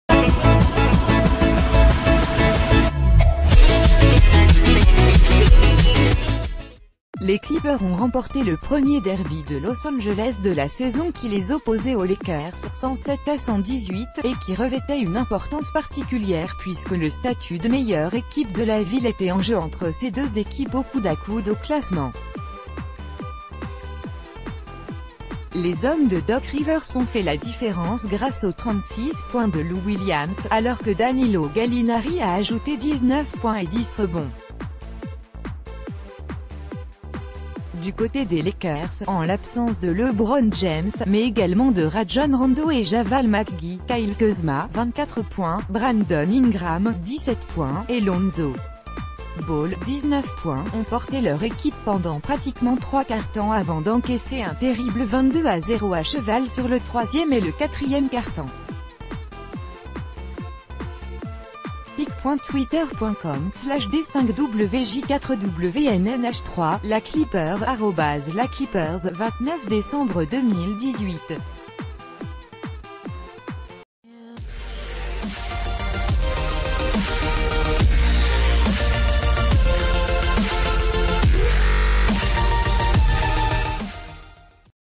Le derby de L.A. pour les Clippers- 24H
Les Clippers ont remporté le premier derby de Los Angeles de la saison qui les opposait aux Lakers (107-118), et qui revêtait une importance particulière puisque le statut de meilleure équipe de la ville était en jeu entre ces deux équipes au coude-à...